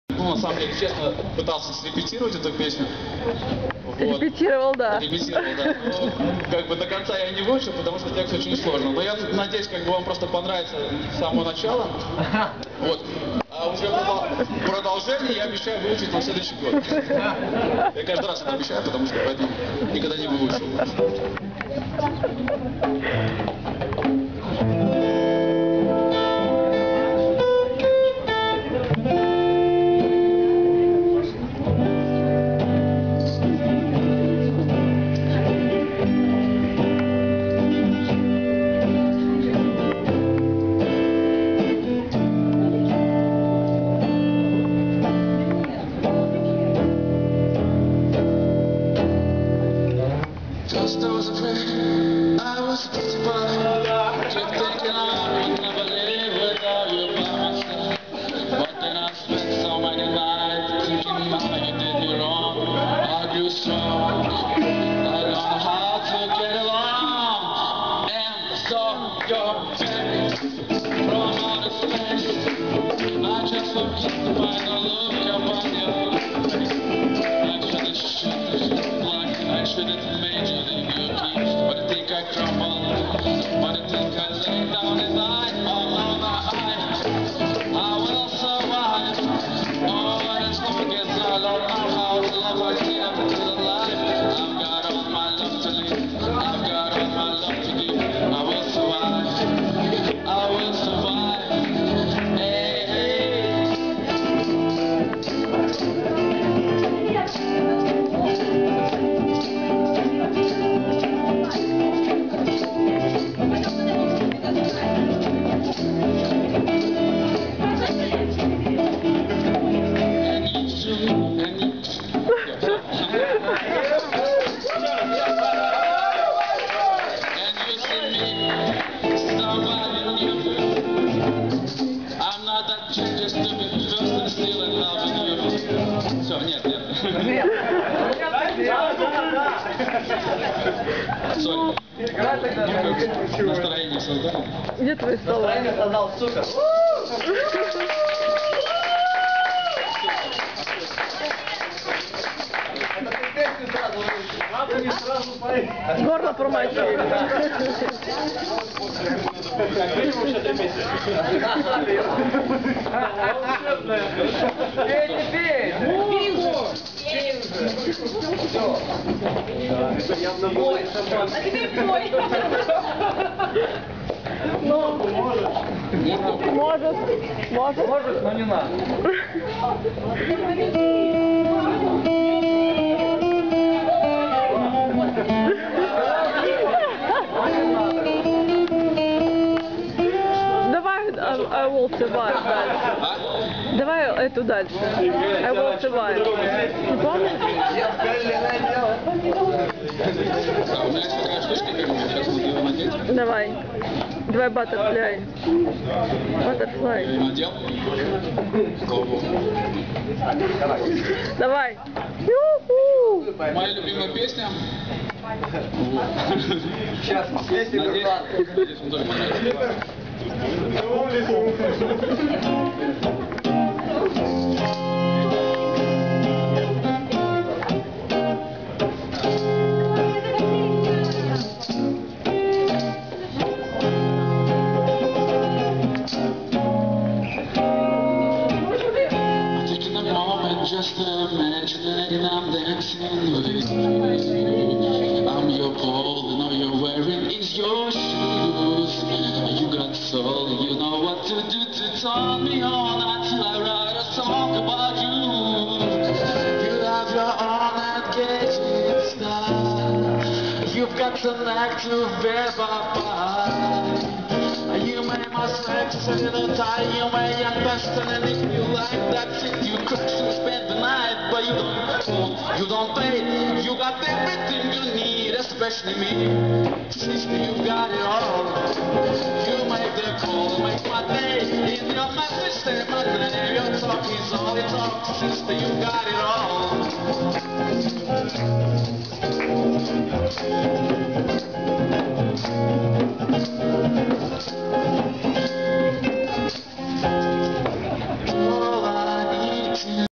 New Year 2009/2010 (Part 2)
This is my success after 15 years. :) I have been playing guitar from 11 years old. Now I'm 26 years old, so my SUCCESS cost me 15 years of my life. Happy New Year 2010. I m happy now. Thank you Aleksej for this party. (By Aleksandro)